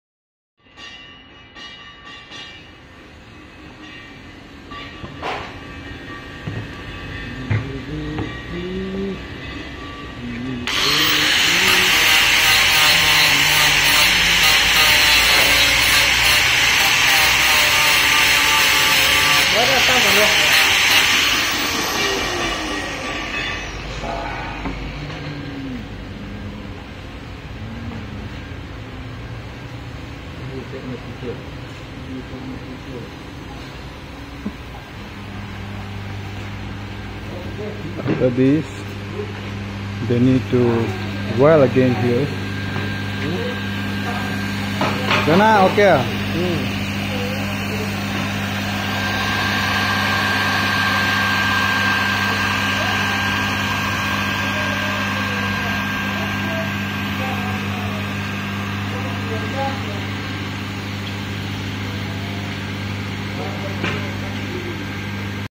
Welding poly pipe T- join and the water line.
Making water line for the classifier.